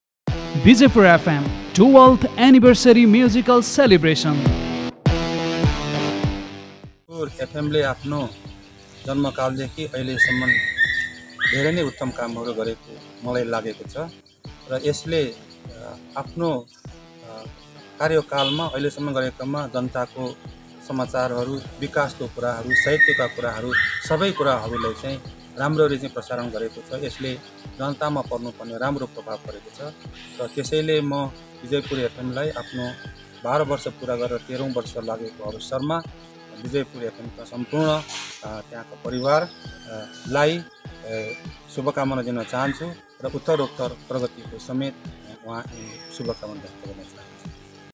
Vijayapur Fm 12th Anniversary musical Celebration Radio live 🔊A AMBIKA SAMBHAHAMPHE LIMBU
Vijayapur Fm 12th Anniversary musical Celebration Radio live 🔊
A MESSAGE OF CONGRATULATIONS
MR.AMBIKA SAMBHAHAMPHE LIMBU